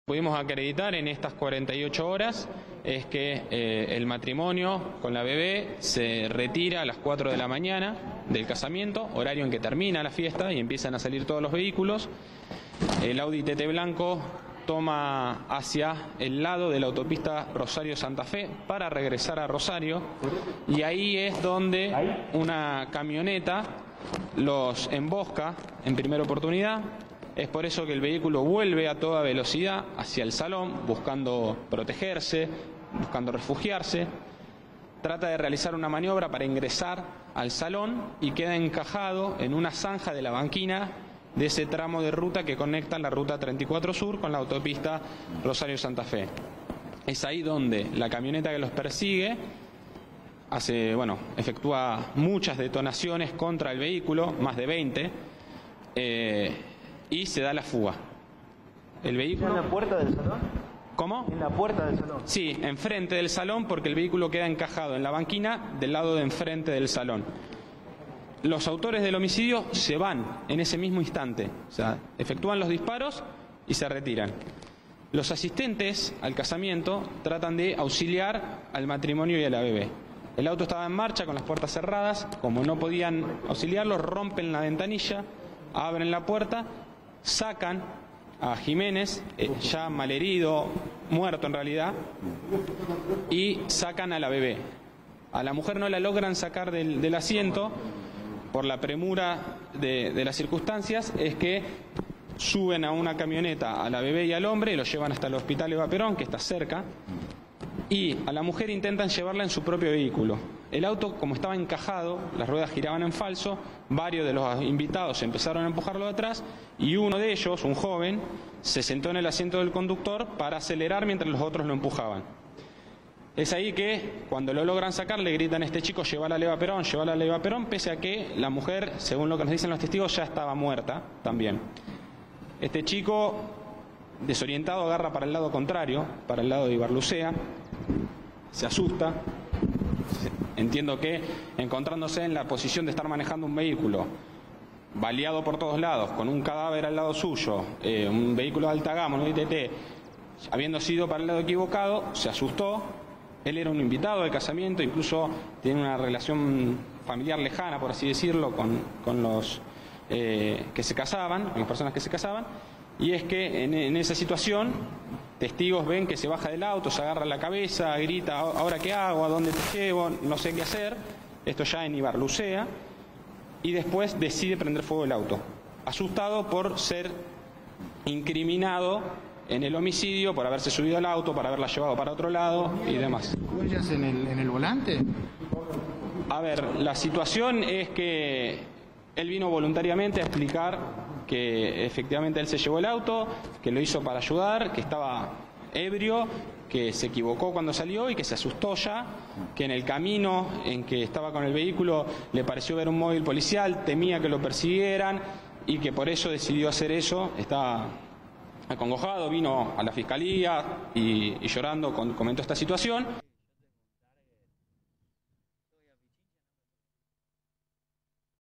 Triple crimen de Ibarlucea: declaró la persona que incendió el auto
El fiscal que investiga el caso confirmó que está identificada y ya declaró la persona que incendió el automóvil en el que apareció el cuerpo de Romero.